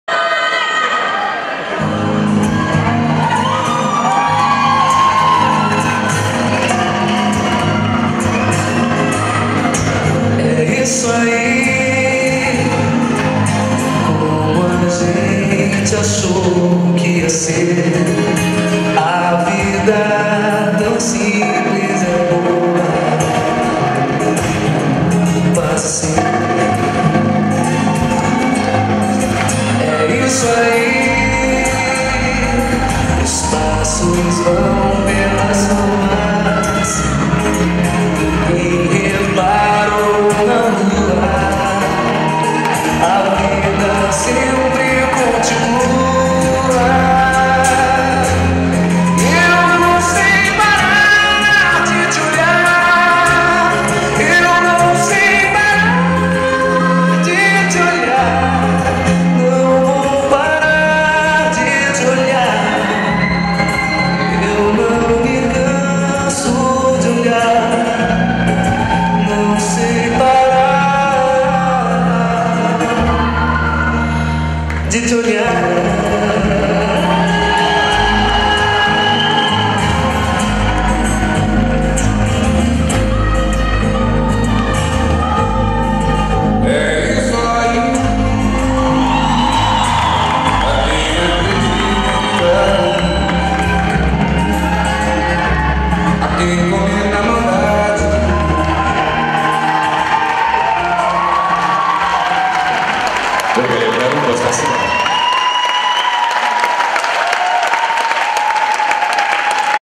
Efimov Ivan - Parkhomenko Yuliya, Final Rumba
Junior-2 Latin
Great Silk Way 2012, Crimea, Sudak.